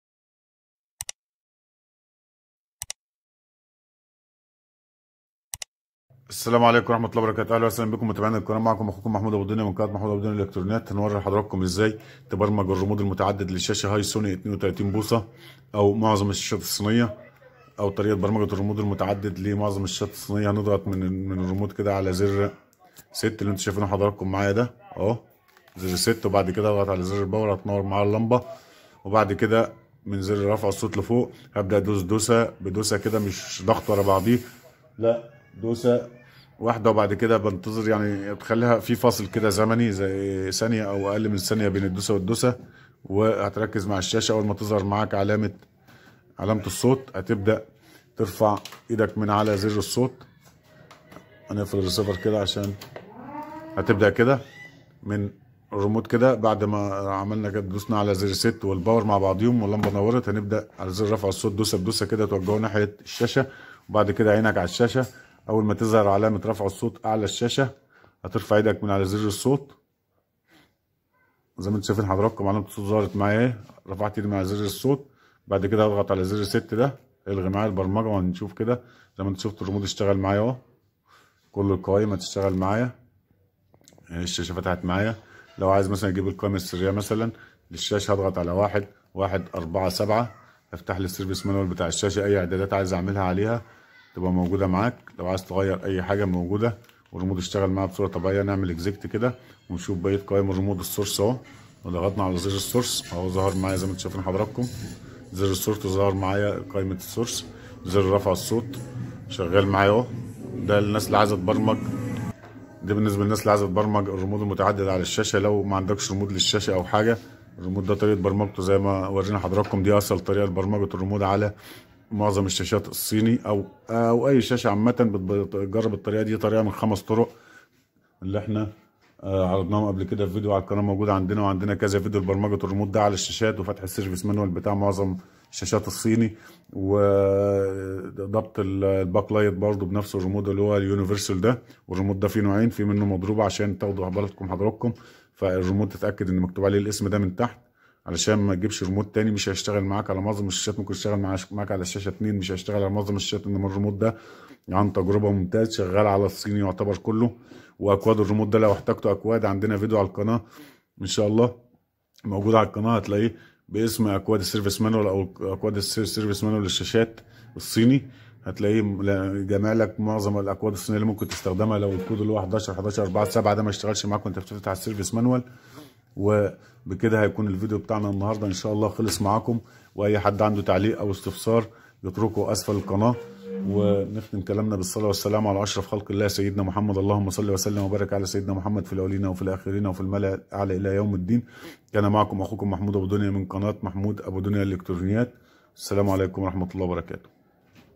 ريموت متعدد شاشة هاي hisony led 32 وفتح السيرفس منول
ضبط ريموت متعدد لشاشة هاي سونيhisony led 32 وفتح السيرفس منول